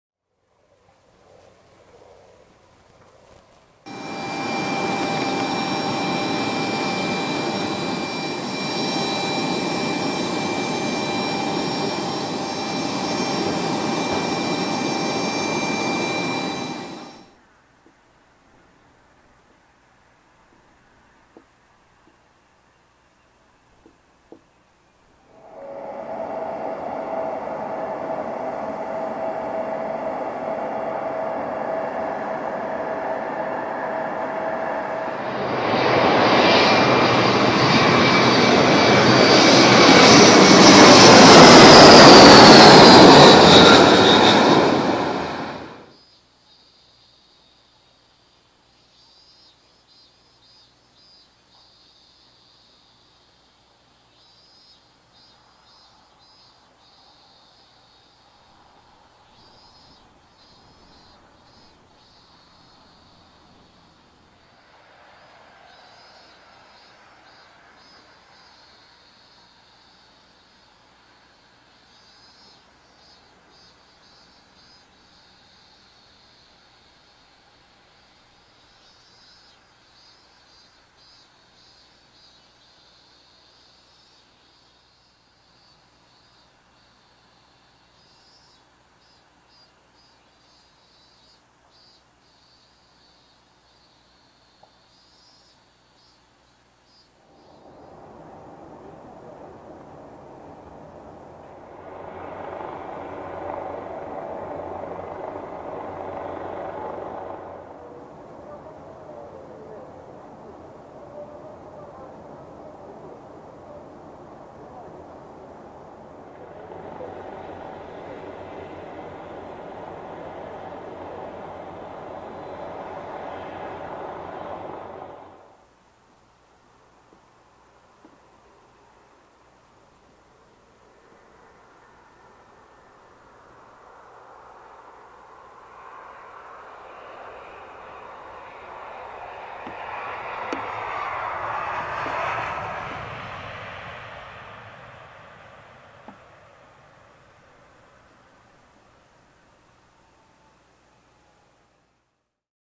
【 日本航空 もう一つの定期便 ★ 夏限定 今年は ?! 松本空港 】JAL ERJ-170 Landing & Arrival at Matsumoto Airport
松本空港の夏限定便JAL(J-AIR)の到着＆着陸動画です。
2020年は、運行されるのでしょうか。
昨年までの思い出を作品にしました。

JAL(J-AIR)Arrival & Landing at Matsumoto Airport.
In Summer, ERJ-170 come the Airport.
Osaka to Matsumoto.